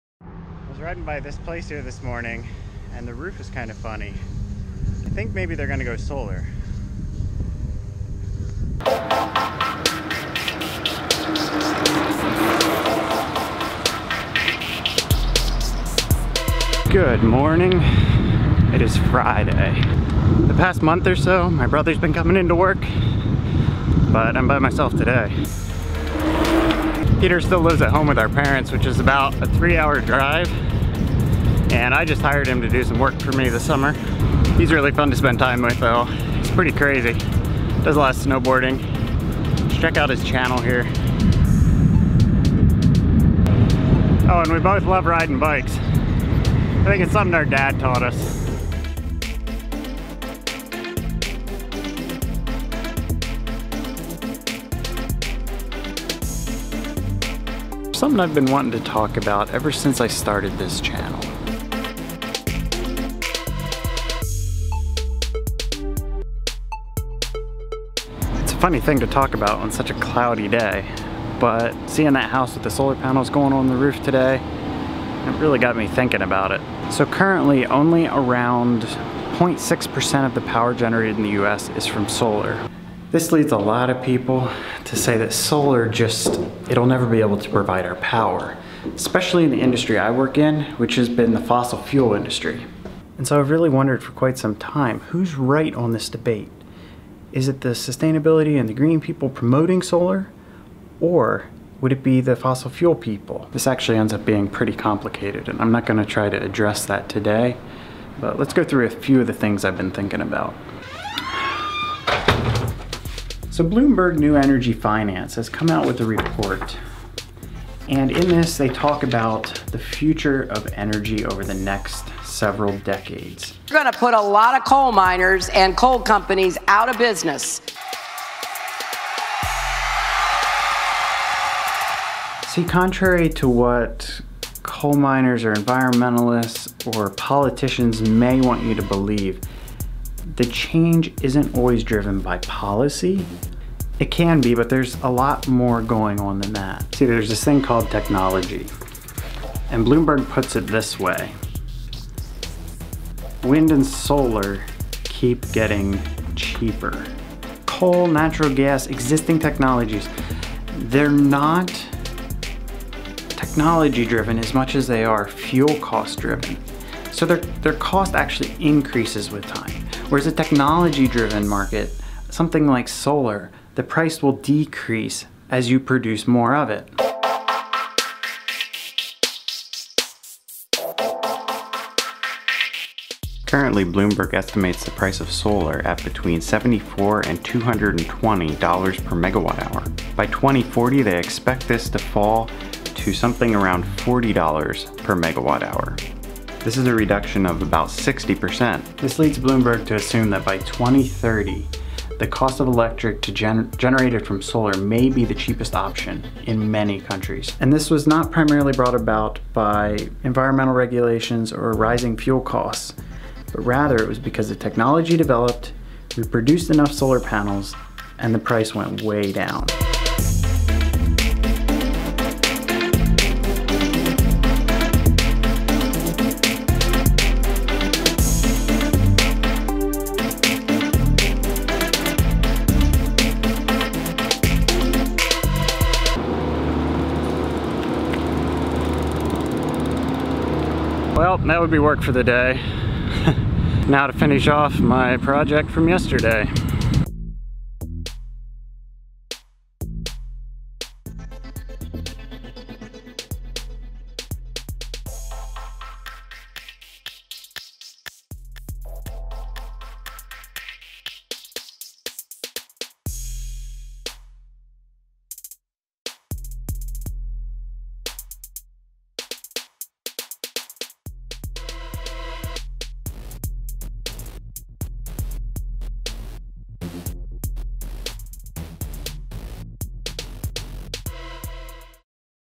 The Future of Solar - The Truth About Solar
What's the truth about solar energy? Is it cost competitive with coal and natural gas? Bloomberg Energy presents their ideas on this in their annual energy review.